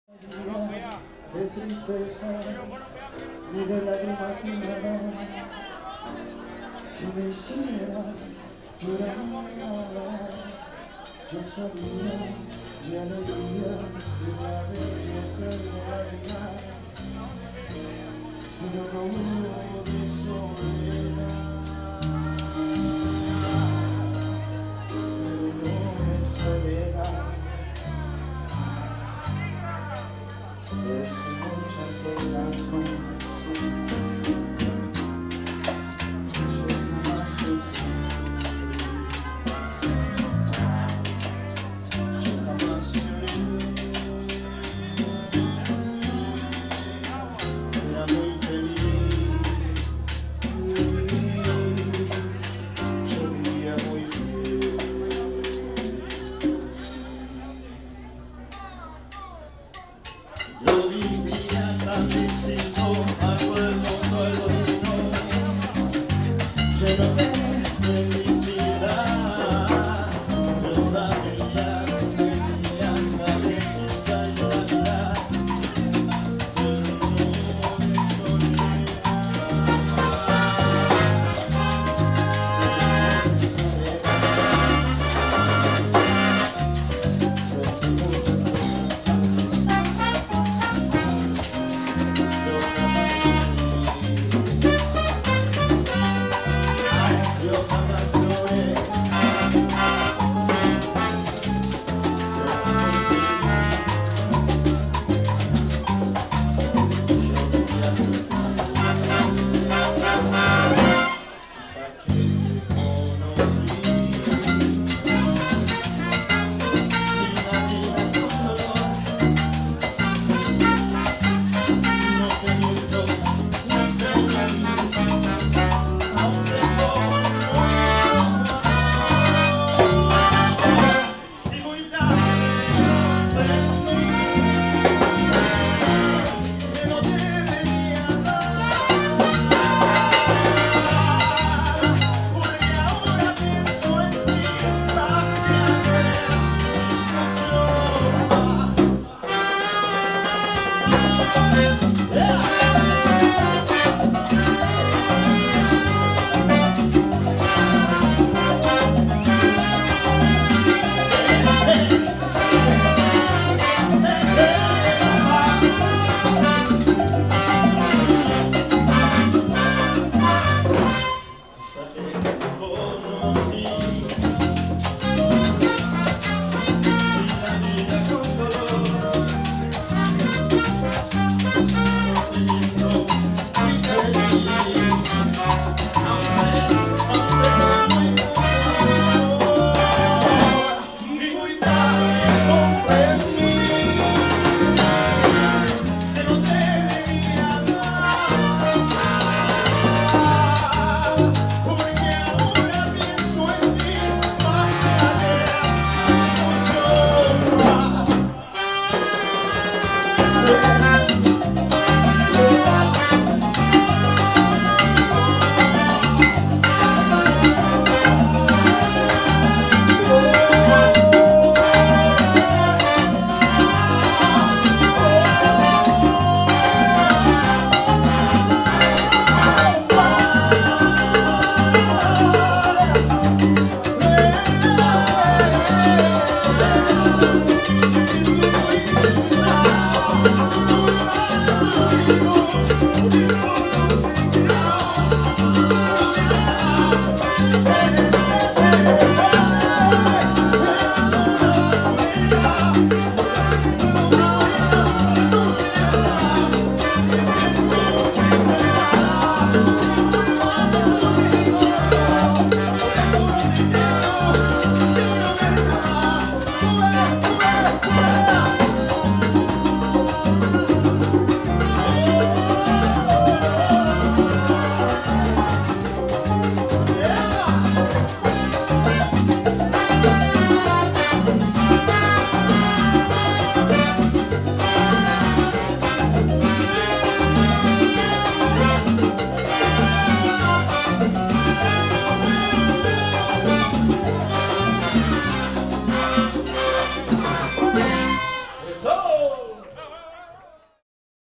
2009 11 18 SL 1704 Tropicana at the Waldorf - Daniel Celis lead vocals
Tropicana the new Sonora Kings performing at the Waldorf Hotel, every Saturday Havana nights. Vancouver, BC Canada November 2009